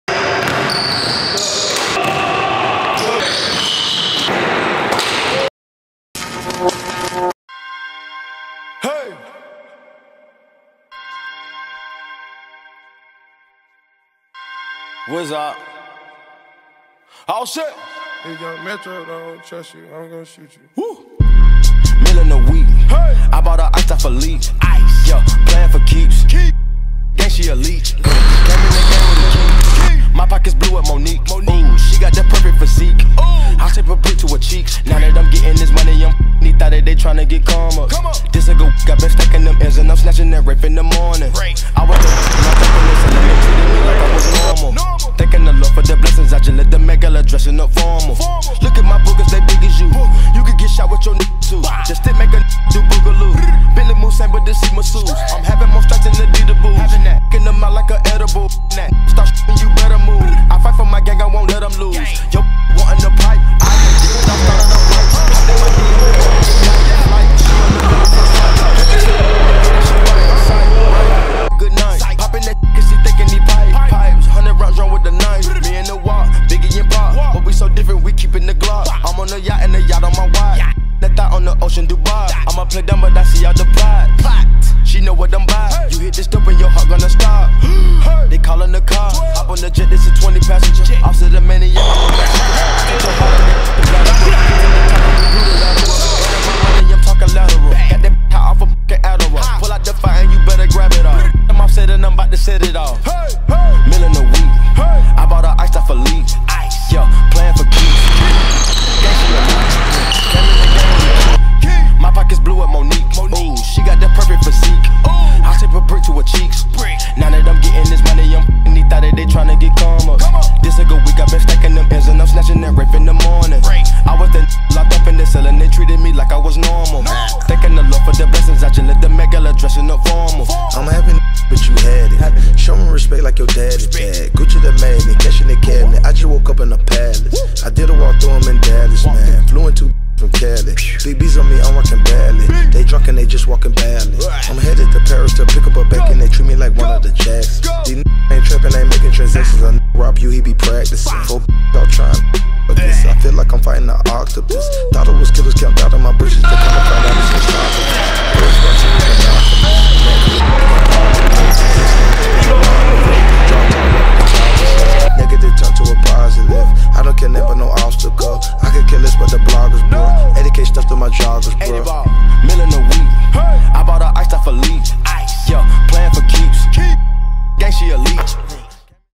Terrell Sanders Workout | "The Forgotten One" (Ep. 3)
Terrell A Sanders
Height 6'5
Weight 190 lbs
11th Grade Combo G
Woodland High School
Greenforest Christian Academy